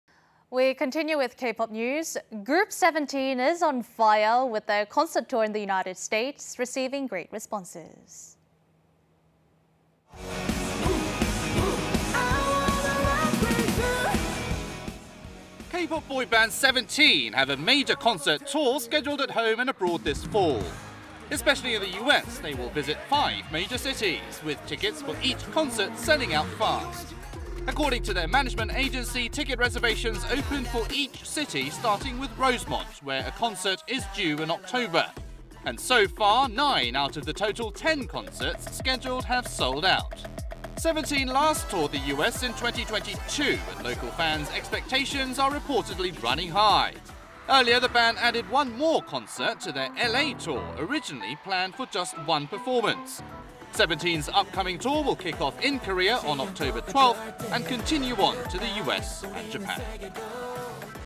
SEVENTEEN'S U.S. TOUR SELLING OUT
We continue with K-POP news. Group Seventeen is on fire with their concert tour in the United States receiving great responses.